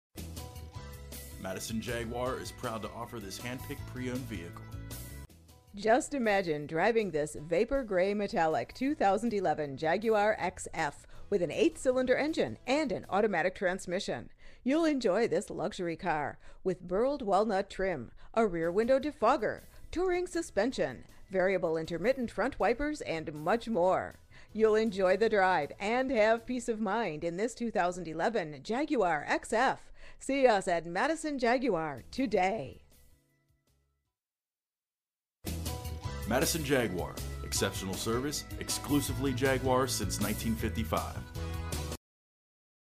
2011 Jaguar XF Madison Jaguar Madison, NJ 07940
Jaguar XF Ready for the very best? Let Madison Jaguar show you what luxury really means. You will love this Vapour Grey Metallic Jaguar Xf Sedan, equipped with an 8 Cylinder engine and an automatic transmission.
MILES: 46,056
VIN:SAJWA0GB4BLR88479

Madison Jaguar
275 Main Street
Madison, New Jersey 07940